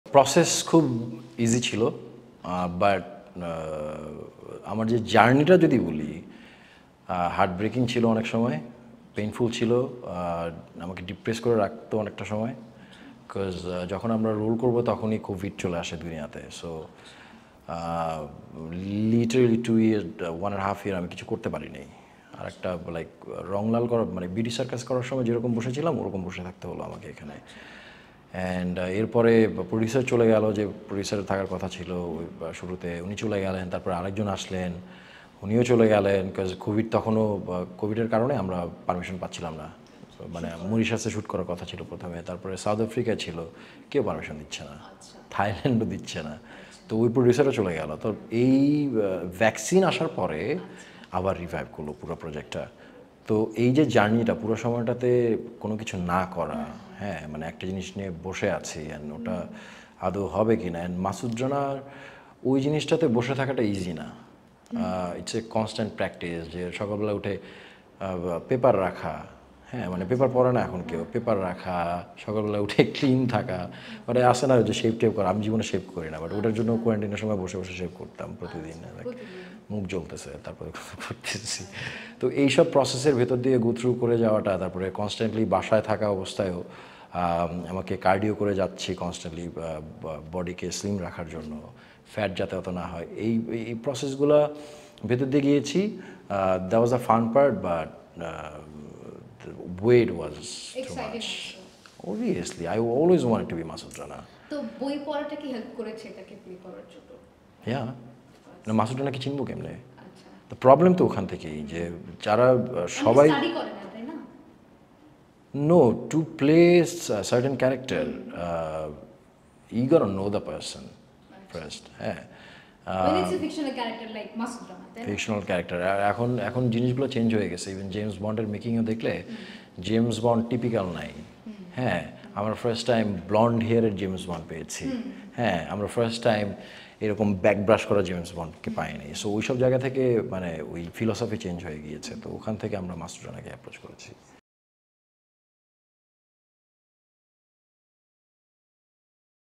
Abm Sumon talks about the film industry & how the pandemic affected the industry
Visit oplly.com and get personalized videos from your favourite celebrities!